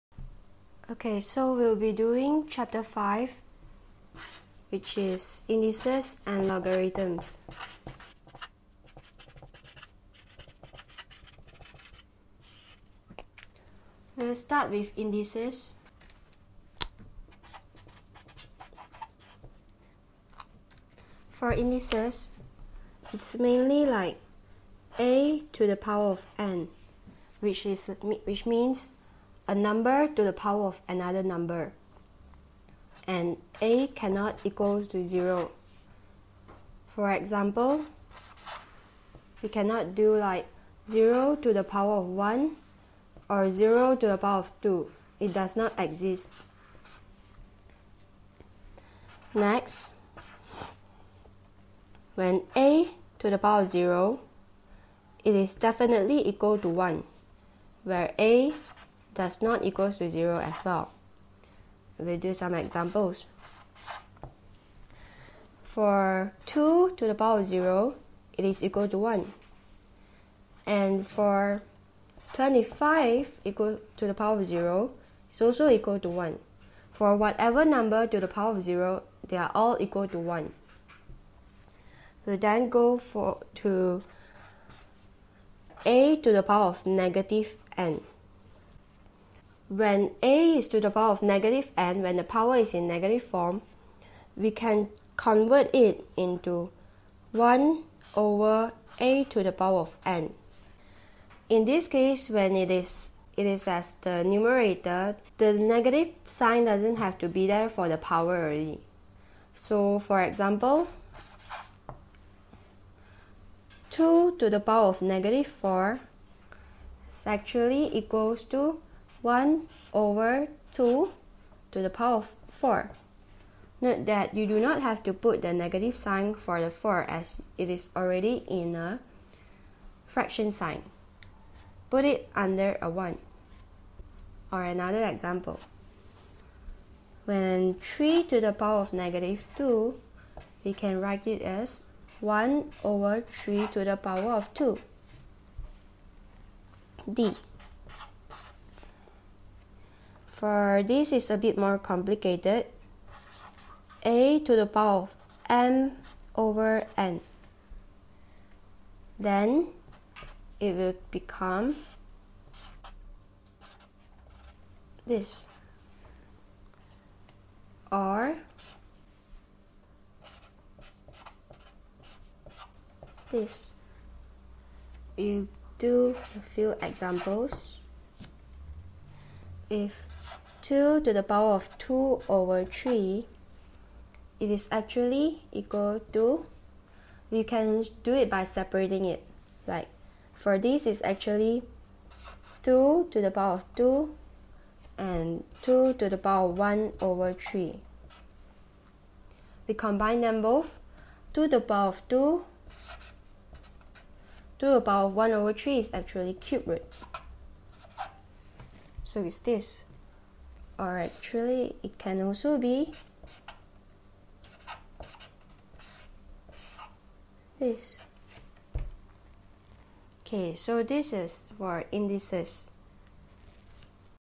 Additional Mathematics - Indices
A brief introduction to what is meant by indices.